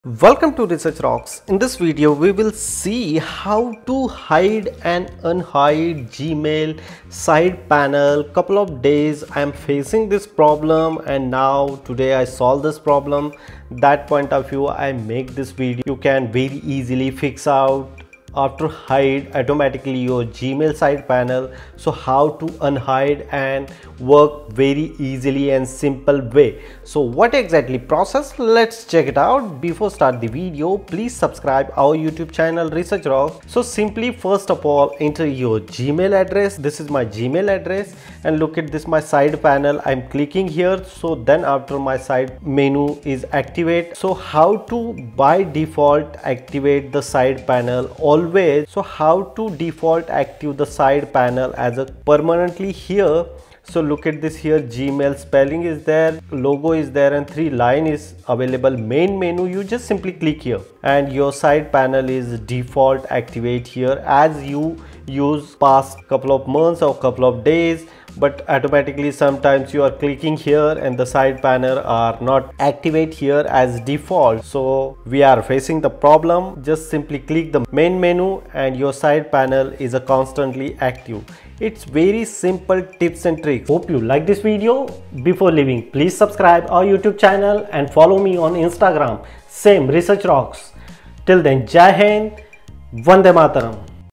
Gmail Side Panels EXPOSED What You Don't Know!
In this video, we'll show you how to easily hide and unhide the Gmail side panels to help streamline your inbox experience. Whether you want to create a cleaner workspace or quickly access Gmail’s main menu, we’ll guide you step-by-step to customize your Gmail layout. Learn this simple trick to boost your productivity and have a clutter-free interface in Gmail.